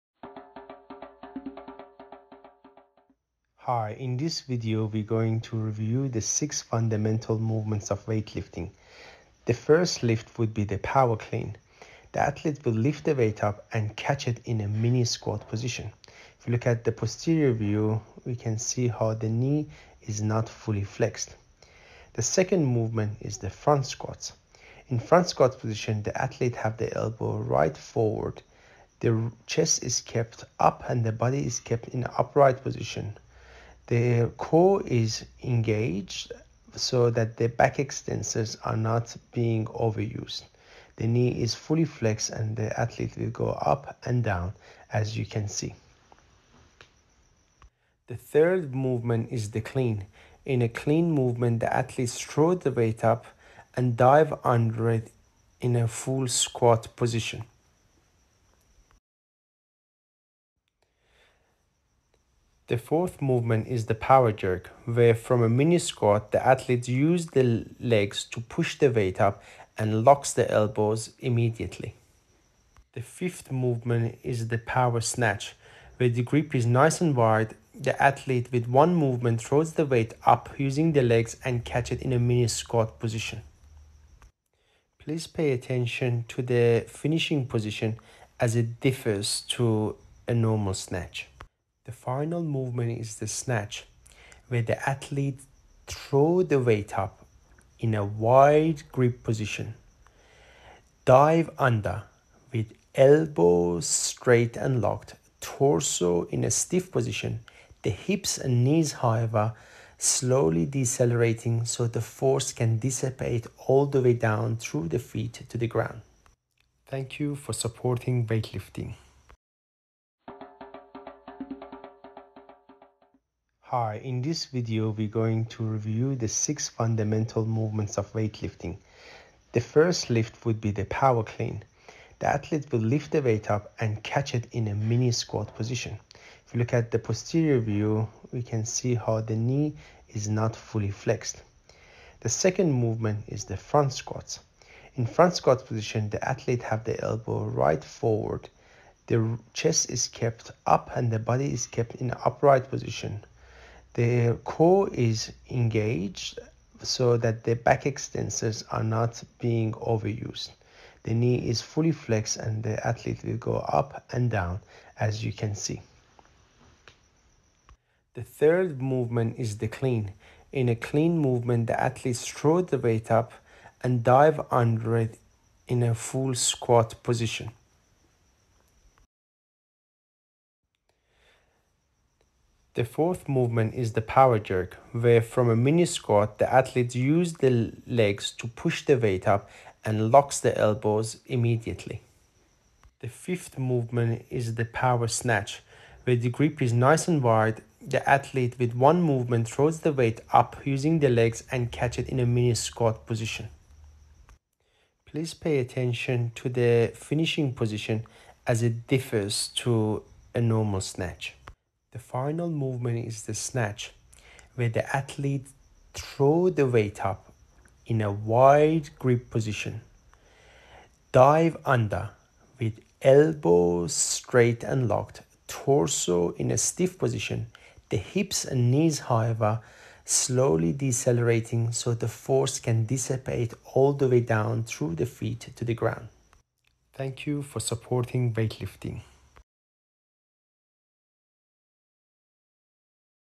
The 6 Fundamental Olympic Lifting By Olympic Physiotherapist Sam Abbaszadeh
Sam is an International Lecturer and Olympic Physiotherapist with a passion to teach complex topics in a simple way.